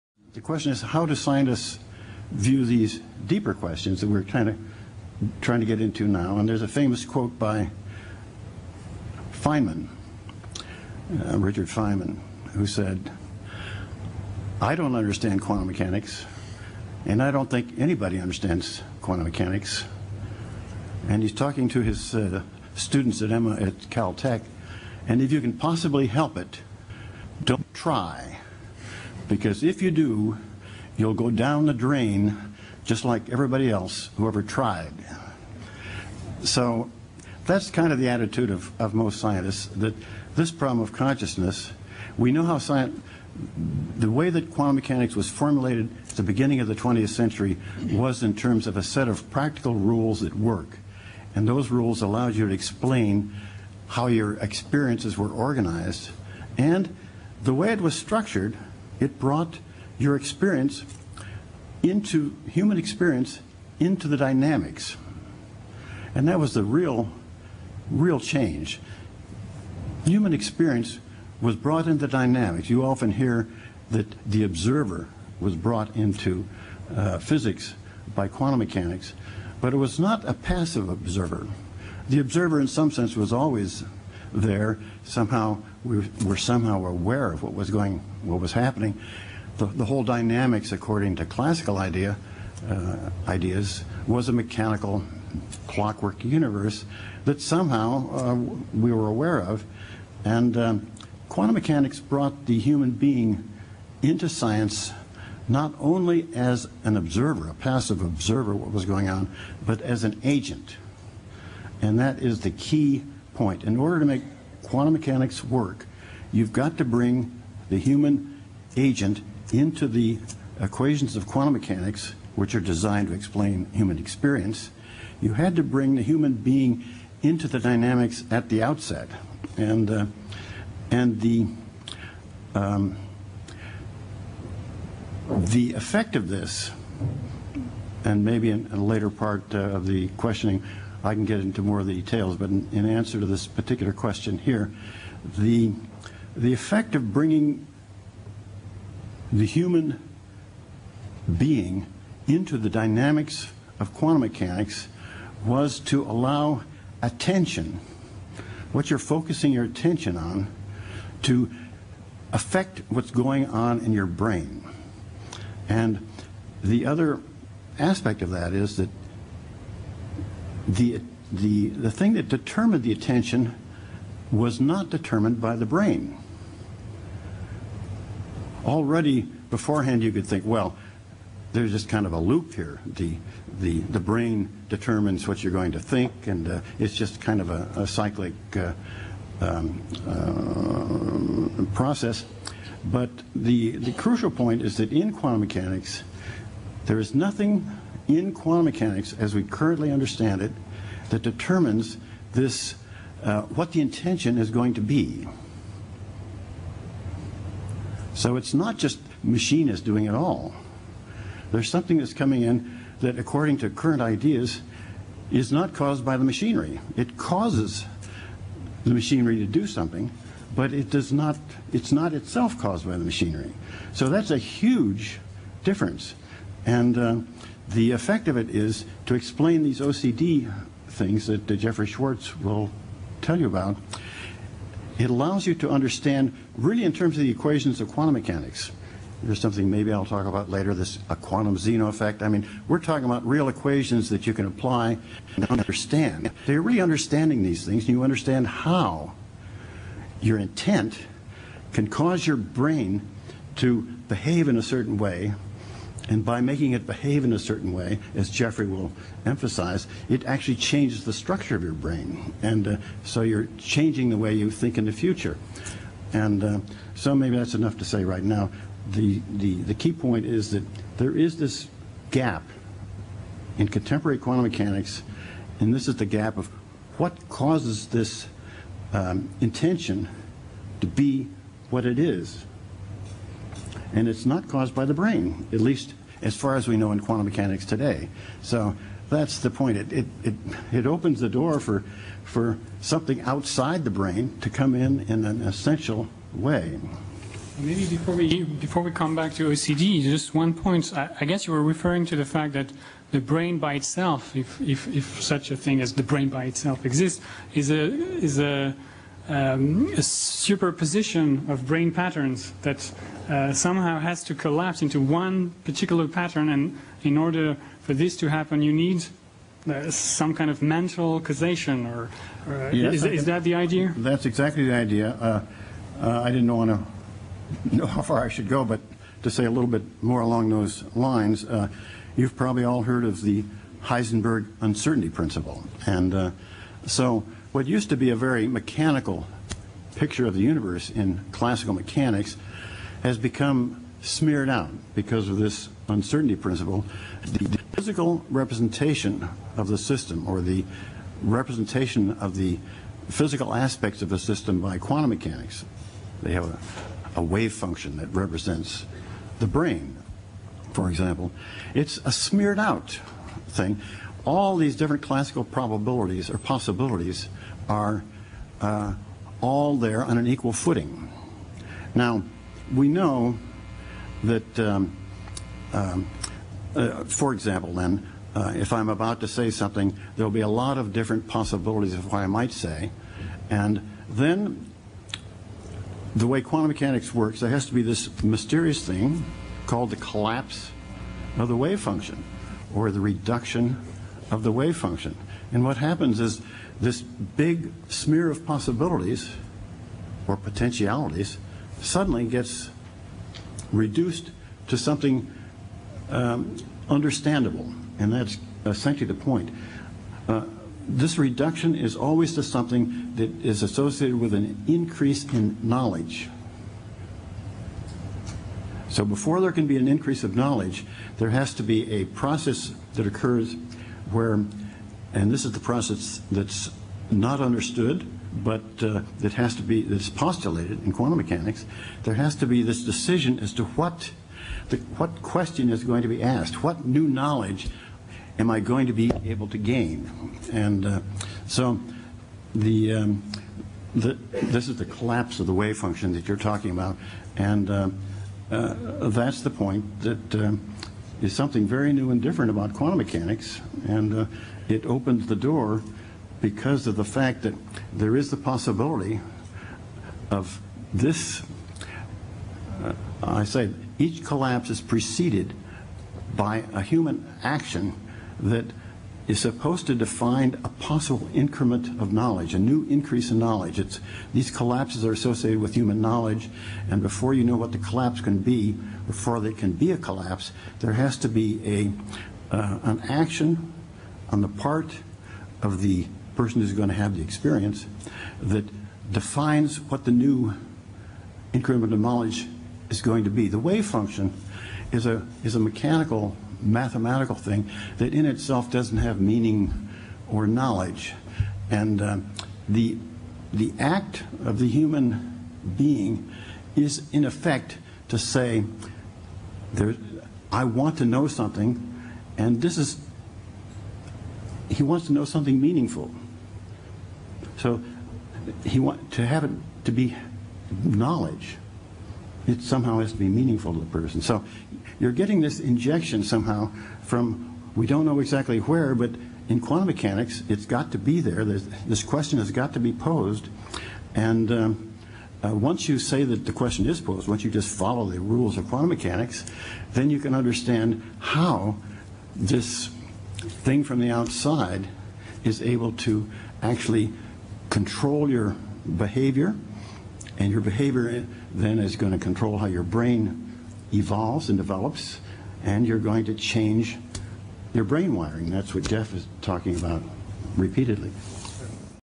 Henry Stapp on how Consciousness affects Quantum Mechanics!
Consciousness causes collapse of the wavefunction and thus actively selects the event observed in the future as an agent choosing among the many possibilities physically represented by the probability distribution imbedded in the quantum wavefunction. This 'agent' restrains what the possible answer can be by simply defining the question in a way that gives meaning to him! In praxis this implies defining the setup of the experiment in the lab or the 'setting' of circumstances in real-life... All intentional choices thereby come to have universal physical effects underpinning the law of karma!